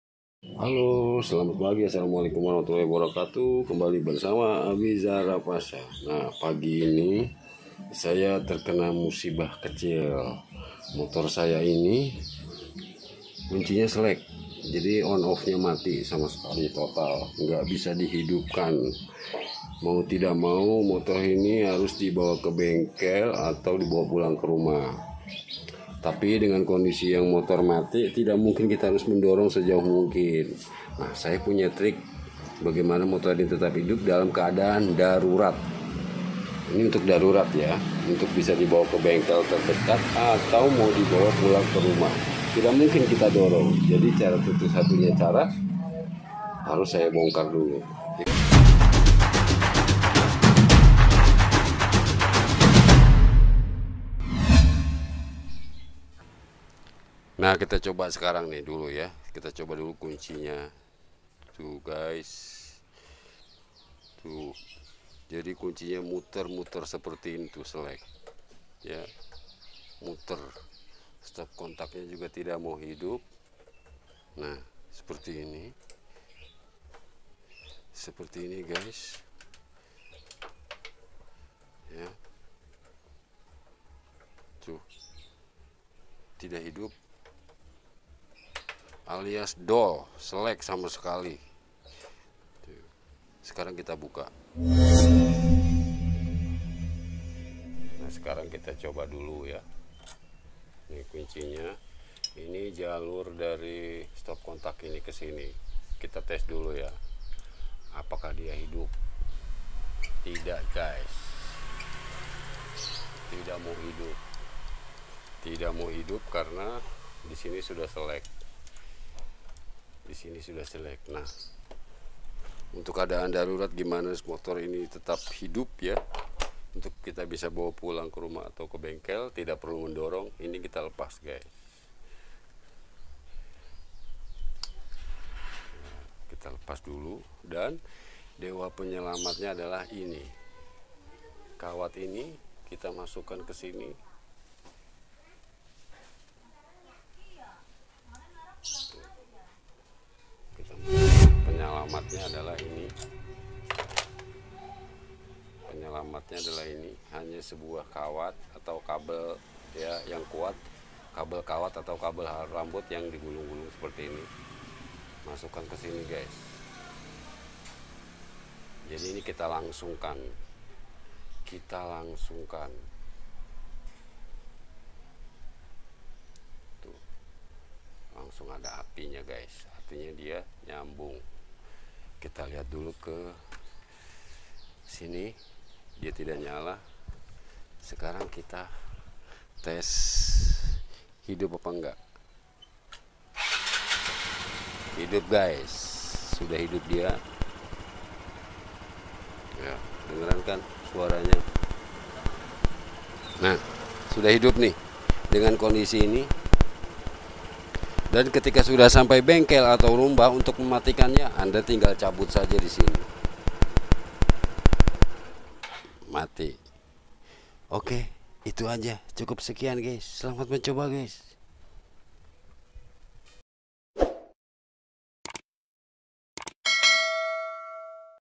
MENGATASI KUNCI MOTOR DOL | SELEC | HILANG
Ketika pulang dari suatu tempat, tiba tiba kunci motor selek dan ngak bisa on, dipaksa eh malah selec alias dol. Kebengkel jauh pulang kerumah oun masih jauh, alhasih saya coba cara ini... dan berhasil sampai rumah..